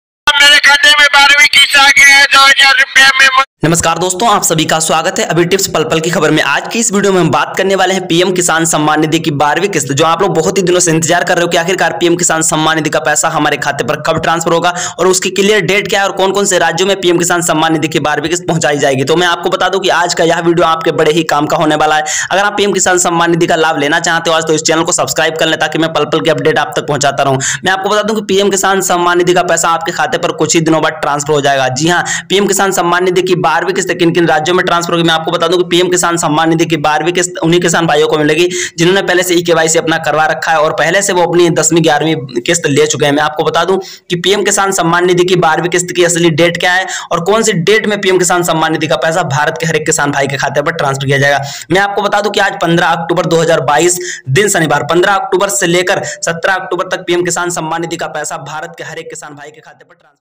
12वीं किस्त ट्रांसफर 1 घंटे बाद ₹2000 || इन किसानों को मिलेगी आज 12वीं किस्त
12वीं किस्त ट्रांसफर 1 घंटे बाद ₹2000 || इन किसानों को मिलेगी आज 12वीं किस्त